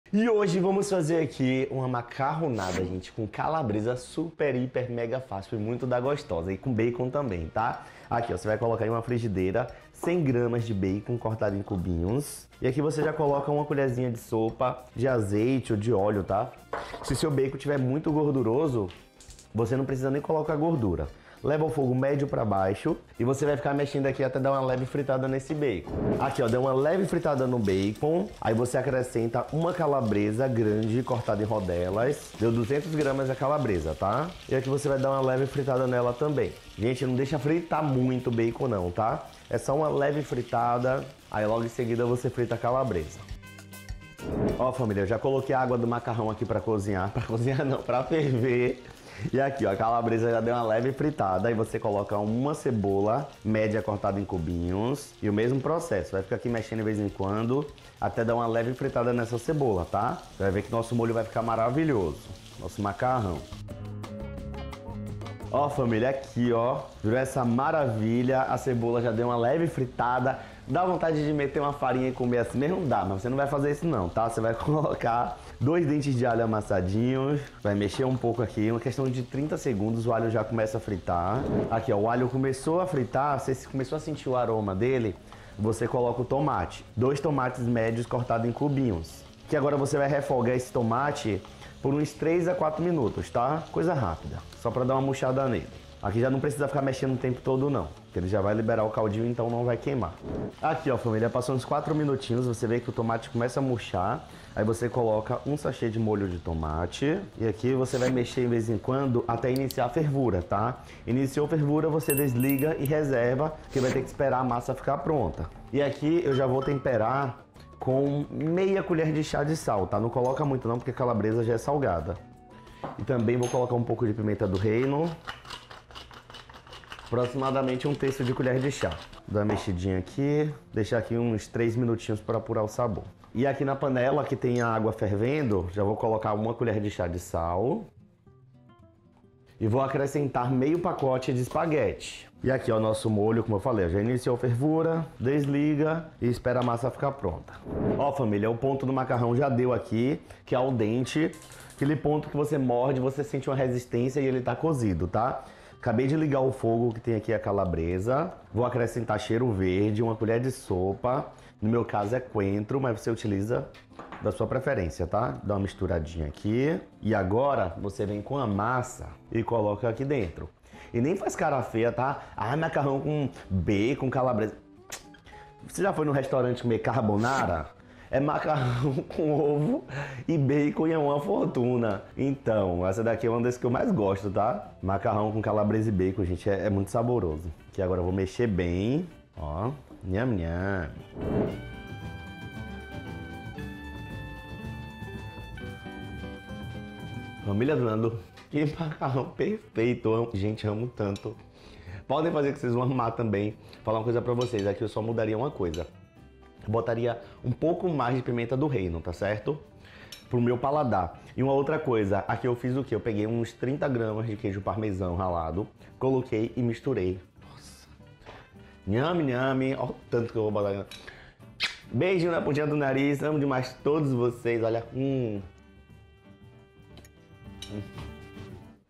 ESSE MACARRÃO ECONÔMICO VAI TE SURPREENDER | Nandu Andrade
Oi família! Hoje vou ensinar uma macarronada com calabresa e bacon, super fácil e muito saborosa!

ESSE MACARRÃO ECONÔMICO VAI TE SURPREENDER

🔴 ingredientes macarrão:
½ pacote de macarrão espaguete (250g)
1 colher de sopa de azeite
1 cebola média picada
2 dentes de alho amassados
2 tomates médios maduros
100g de bacon picado
1 calabresa (200g)
1 sachê de molho de tomate (300g)
½ colher de chá de sal
⅓ de colher de chá de pimenta-do-reino
1 colher de sopa de cheiro verde
30g de queijo parmesão ralado na hora